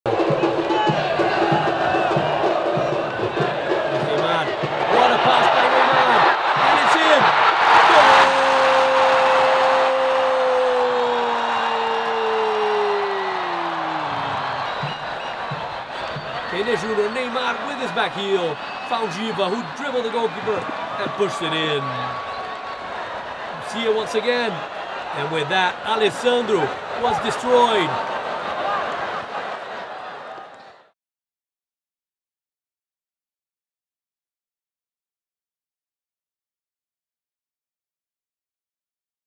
Neymar's great piece of skill - Flamengo v Santos
Flamengo v Santos - Giva scores after clever flick from Neymar.